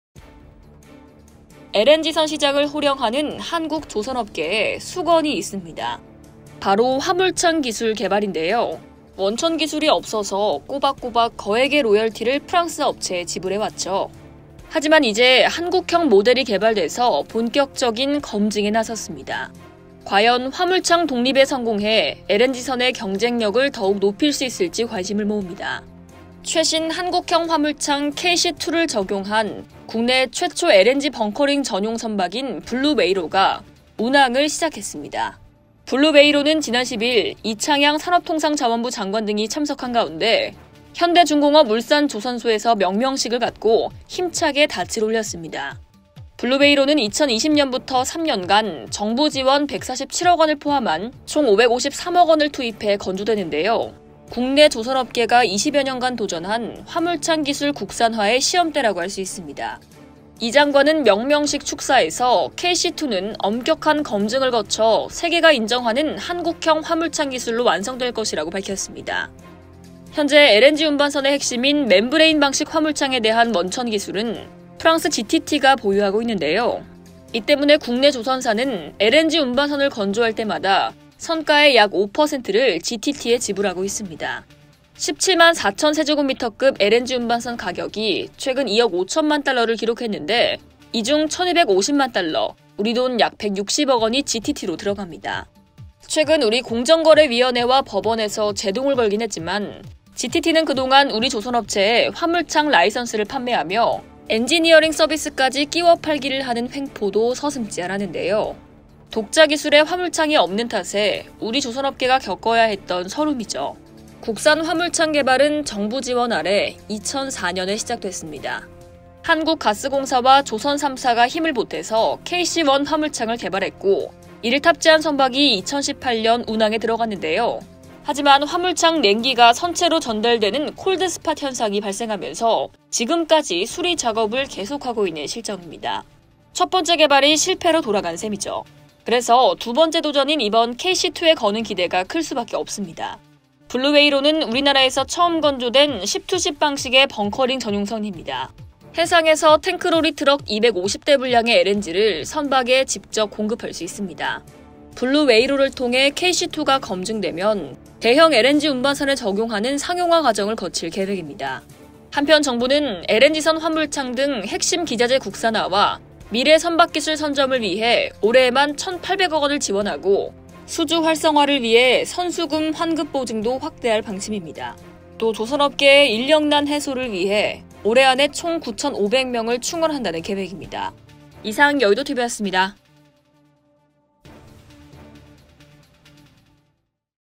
[여의도튜브] “더 이상 뜯기지 않겠다”韓 LNG선 비장한 출항 / 머니투데이방송
LNG선 시장을 호령하는 한국 조선업계에 숙원이 있습니다. 바로 화물창 기술 개발인데요. 원천기술이 없어 꼬박꼬박 거액의 로열티를 프랑스 업체에 지불해왔죠. 하지만 이제 새로운 한국형 모델이 개발돼 본격적인 검증에 나섰습니다. 과연 ‘화물창 독립’에 성공해 LNG선의 경쟁력을 더욱 높일 수 있을지 관심을 모읍니다.

최신 한국형 화물창 KC-2를 적용한 국내 최초 LNG 벙커링 전용 선박인 블루웨일호가 운항을 시작했습니다. 블루웨일호는 지난 10일 이창양 산업통상자원부 장관 등이 참석한 가운데 현대중공업 울산조선소에서 명명식을 갖고 힘차게 닻을 올렸습니다.

아나운서: 선소연
영상콘텐츠: 김수진
기사콘텐츠: 우한나
기획: 강인형
총괄: 정미경
제작: MTN 머니투데이방송